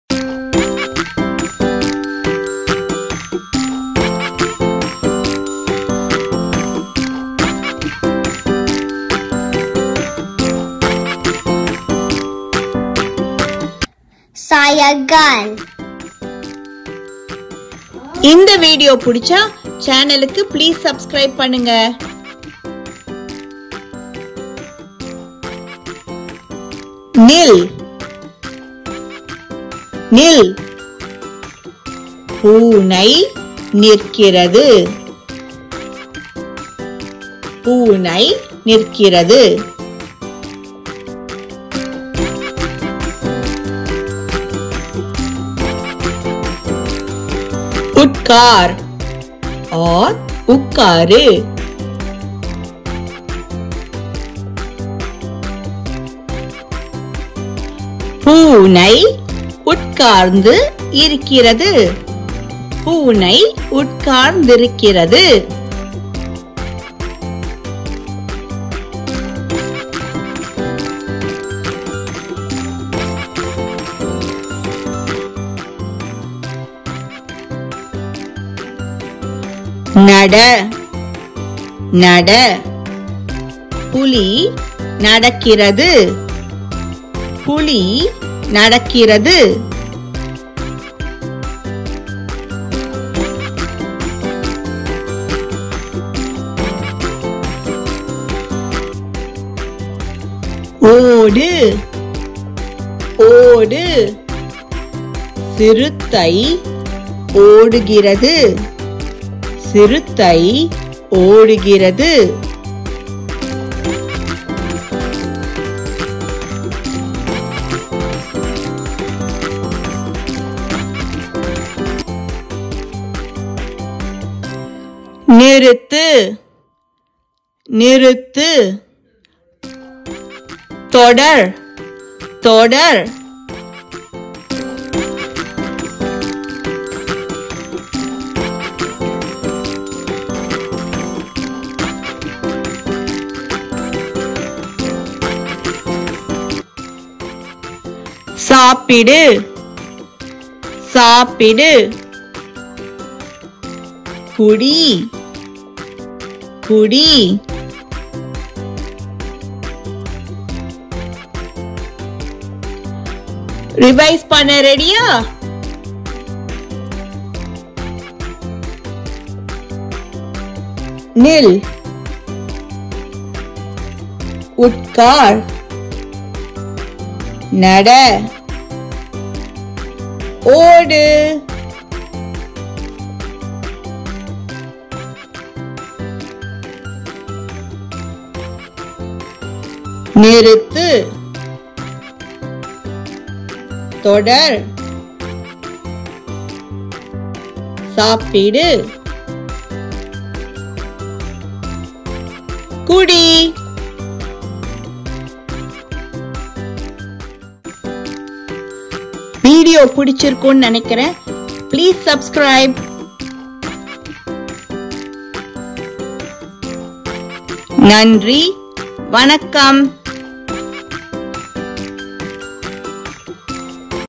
குழந்தைகளுக்கான வீடியோ செயல்கள் | Learn Action in Tamil and English for Kids 💗
Part 1 - குழந்தைகளுக்கான எளிய வீடியோ செயல்கள்
Hi friends, this video contains learning action names in English and Tamil for kids and children - குழந்தைகளுக்கான எளிய வீடியோ செயல்கள்.

Chapters:
00:00 Introduction
00:17 Subscribe
00:23 Chapter 1 action words and sentences
01:12 Chapter 2 action words and sentences
02:00 Chapter 3 action words and sentences
02:40 Let's revise
03:27 Subscribe and thanks